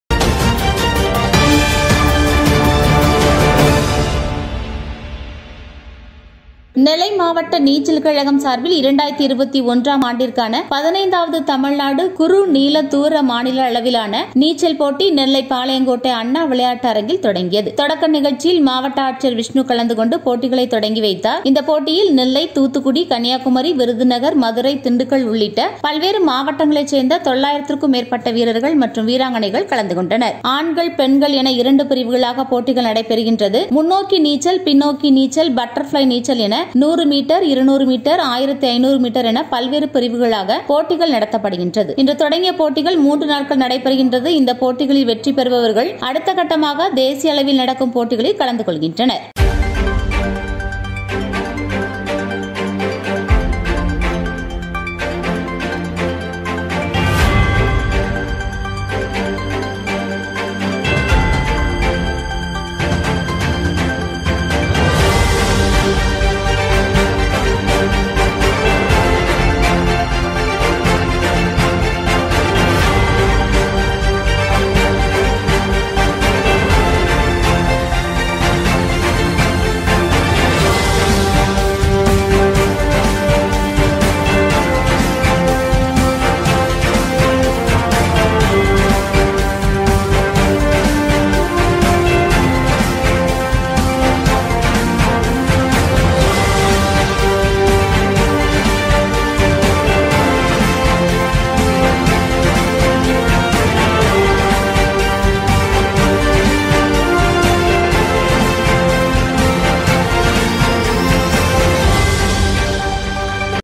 15- வது தமிழ்நாடு குறுநீள தூர மாநில அளவிலான நீச்சல் போட்டி
15- வது தமிழ்நாடு குறுநீள தூர மாநில அளவிலான நீச்சல் போட்டி நெல்லையில் தொடங்கியது. மூன்று நாட்கள் நடக்கும் இந்த  போட்டியில் மாநிலத்தின் பல்வேறு பகுதிகளில் இருந்து வீரர்கள் மற்று வீராங்கனையில் பங்கேற்று உள்ளனர்.